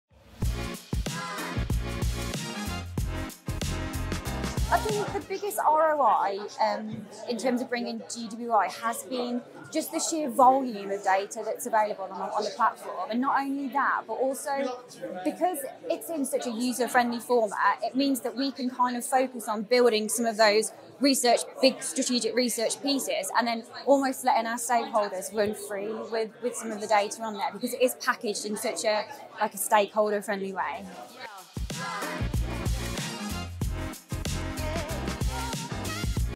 Gymshark X GWI - The value of instantly accessible insights
The team at athletic wear brand Gymshark talk about driving ROI with GWI’s consumer research platform by having a wealth of data at their fingertips in a super user friendly format.